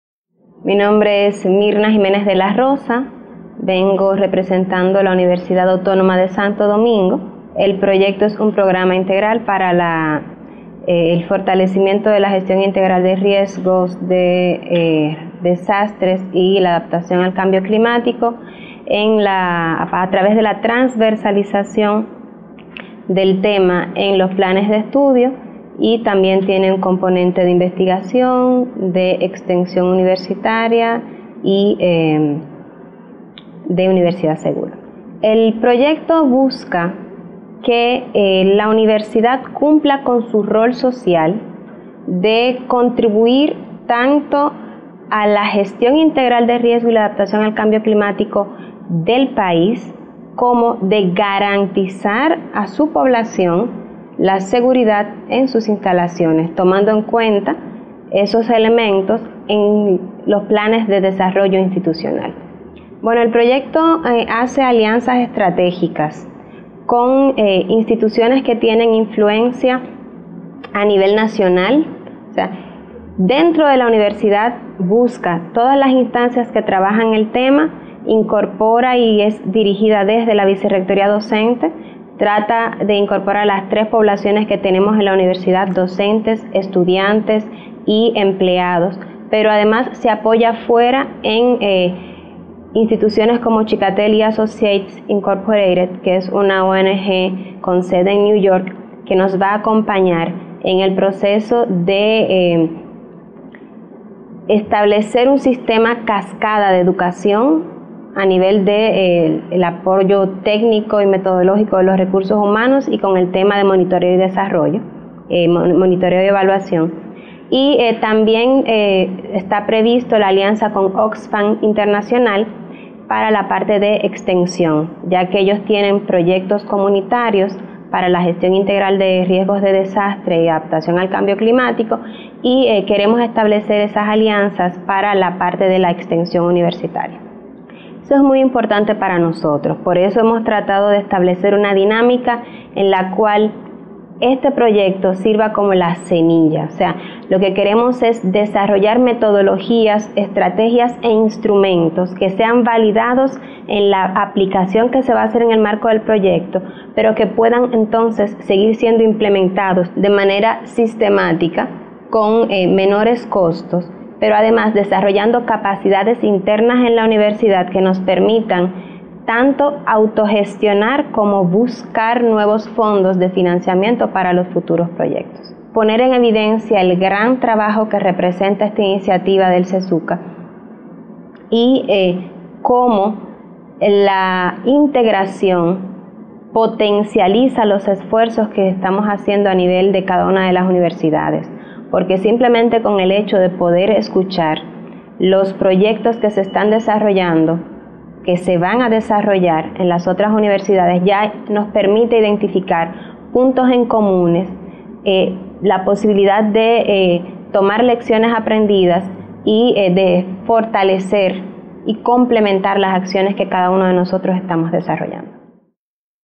Mirna de la Rosa, Universidad Autónoma de Santo Domingo (UASD)
Proyecto de Fortalecimiento de Gestión Integral del Riesgo de Desastres (GIRD) y la Adaptación al Cambio Climático (ACC) en la de la Universidad Autónoma de Santo Domingo (UASD): Componente de Transversalización del GIRD y ACC en los Planes de Estudio